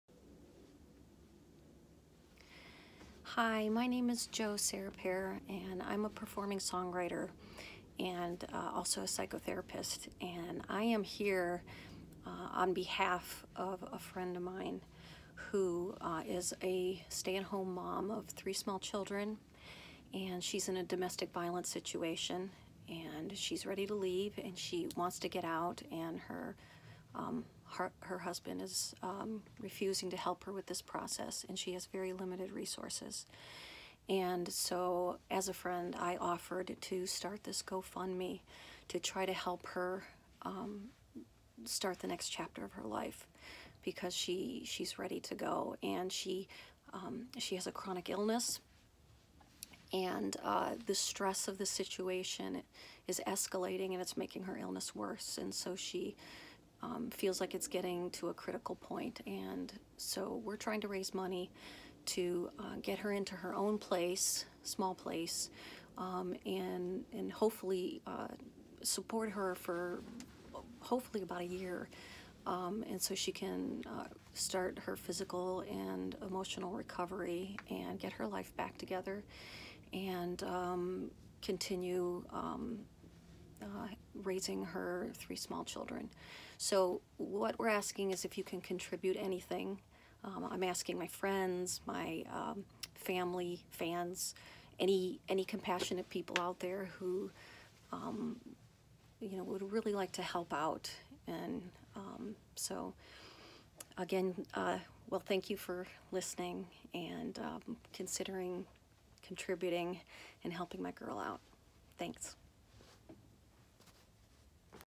Operation Safe Home
Please help my friend and her 3 small kids move out of a domestic violence situation. She wishes to stay anonymous for her safety.